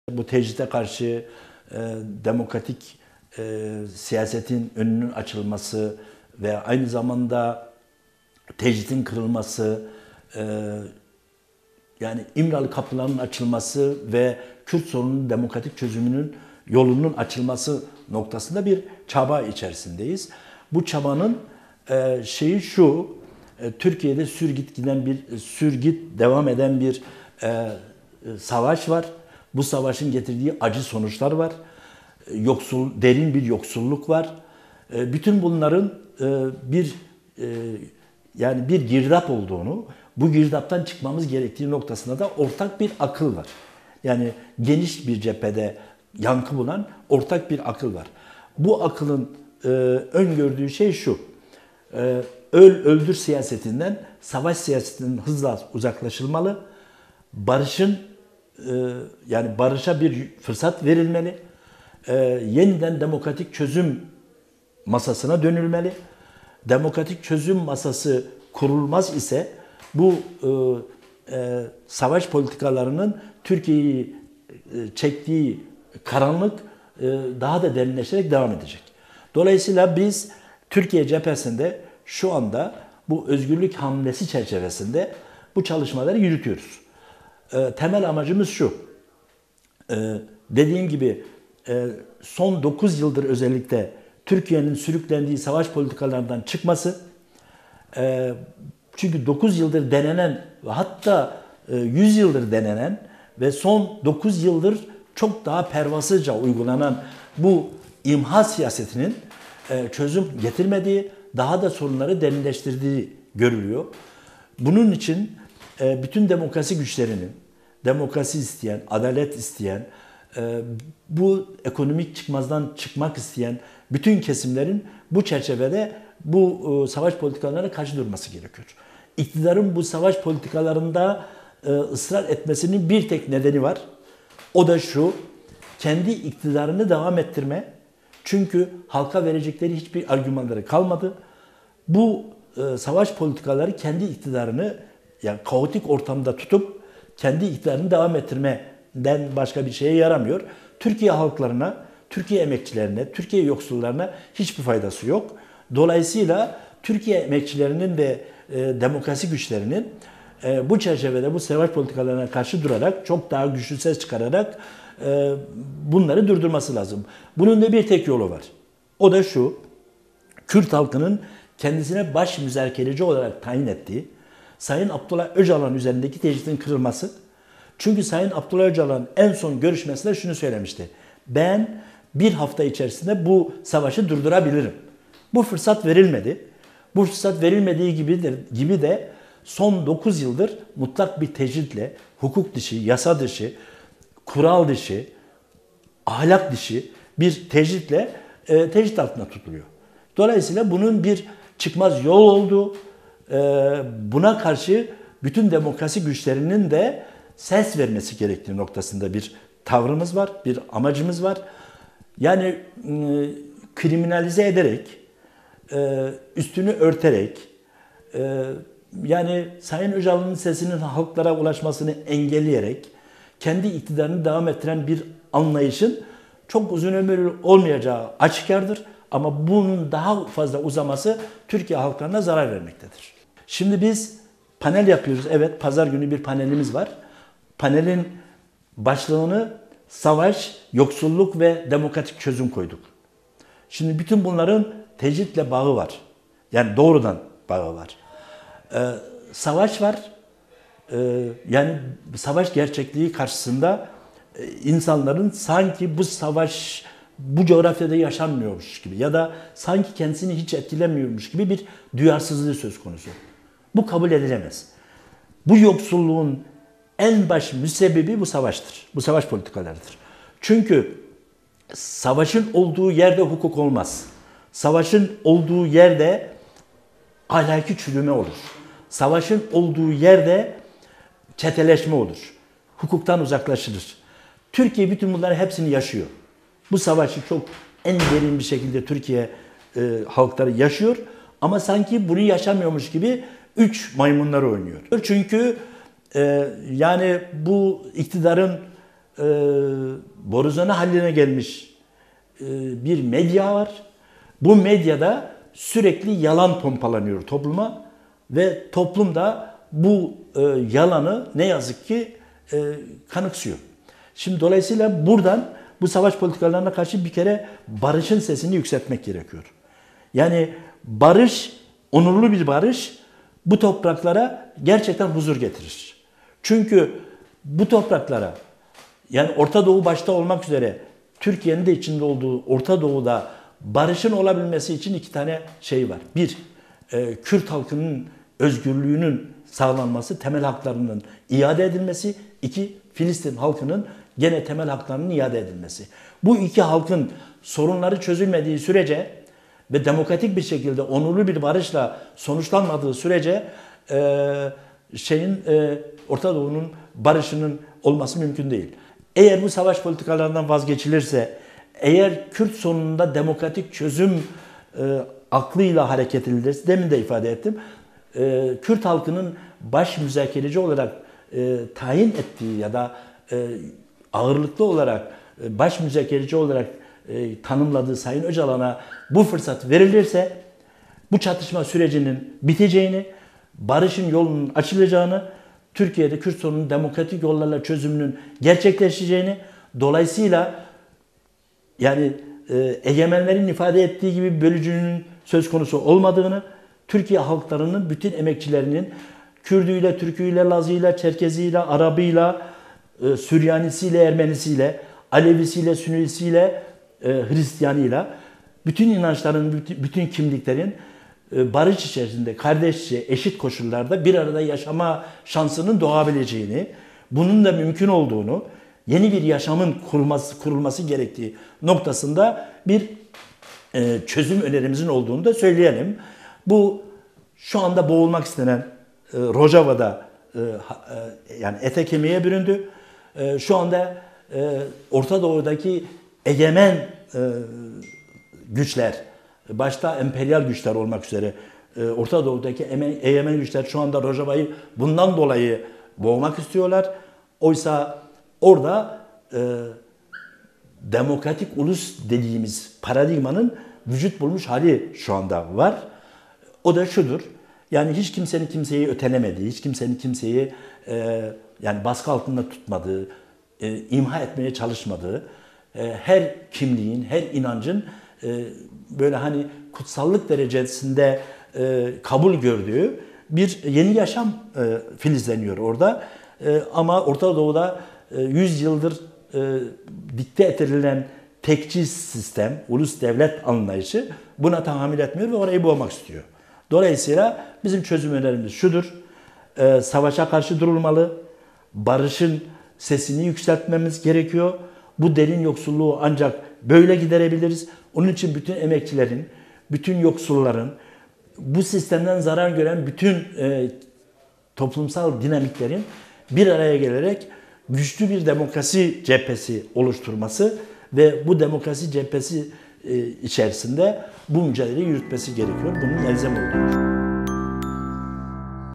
'SAVAŞ VE ÇÖZÜM' PANELİNİN HAZIRLIKLARI SÜRÜYOR: SORUNLARI BİRLİKTE ÇÖZELİM
İstanbul'da yapılacak “Savaş, yoksulluk ve demokratik çözüm” konulu panelin hazırlık çalışmalarında yer alan DEM Partili Attila Özdoğan, "Tecrit ve bununla bağlantılı olan yoksulluk hepimizin sorunu. Gelin Türkiye'yi bu girdaptan çıkaralım" dedi.